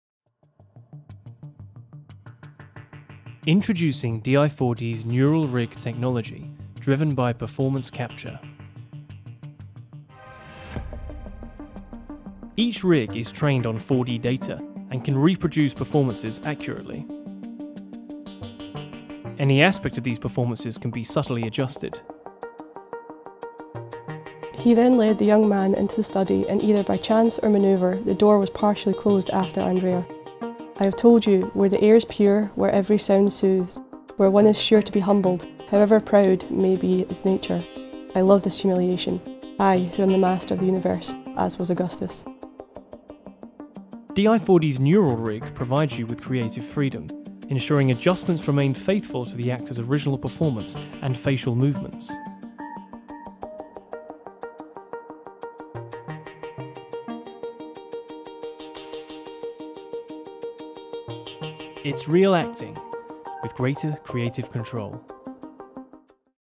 Introducing DI4D's neural rig
Introducing our new, highly efficient rigging solution to deliver facial animation that reproduces every nuance of a real-life actor. The solution uses machine learning to analyse a few minutes of 4D facial performance data to build an actor-specific facial animation rig.

Our neural rig is particularly effective when combined with our PURE4D 4D facial performance capture solution. 4D “geometry cache” facial animation, such as that produced by PURE4D, can be solved accurately onto the neural rig, ensuring the resulting rig-based animation remains faithful to the original captured performance while allowing adjustments to be made where required.